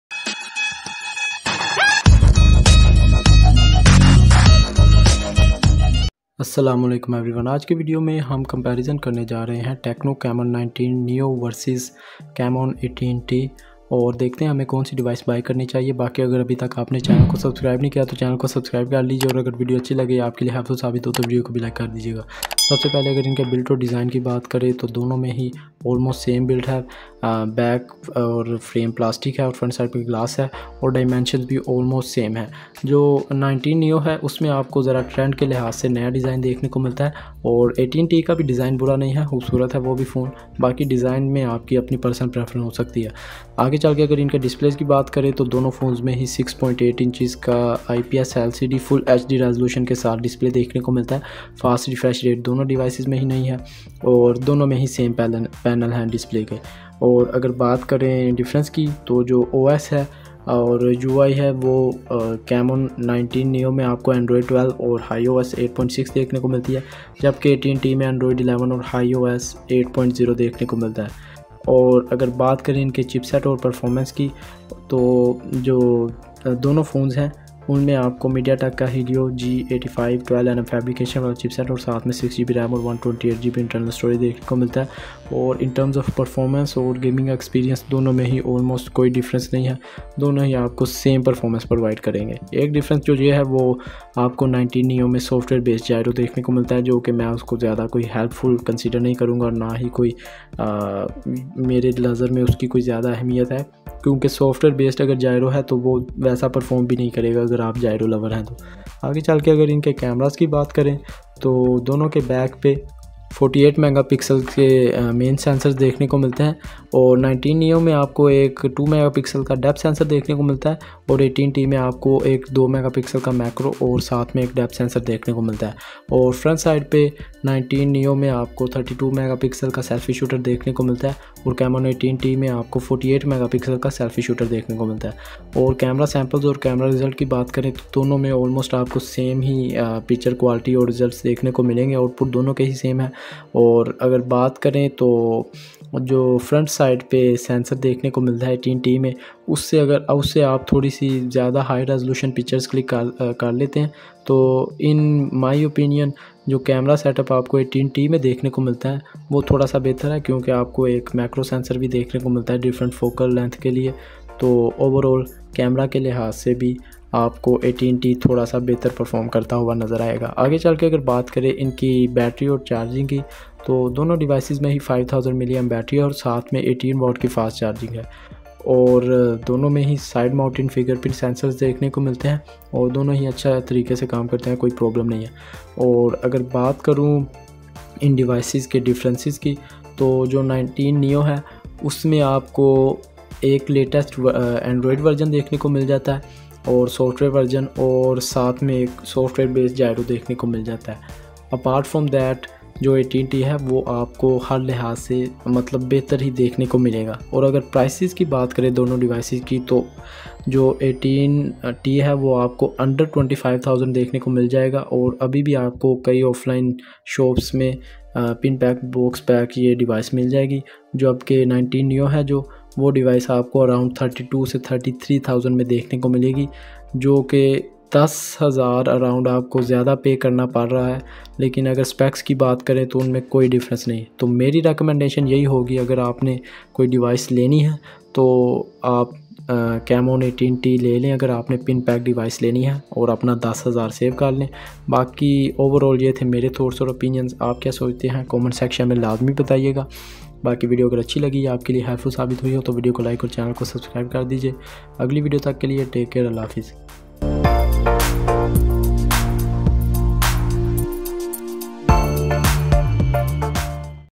Tecno Camon 19 Neo vs Tecno Camon 18t Comparison!
Hello Friends
                 Today I am going to show you Tecno Camon 19 Neo vs Tecno Camon 18t Comparison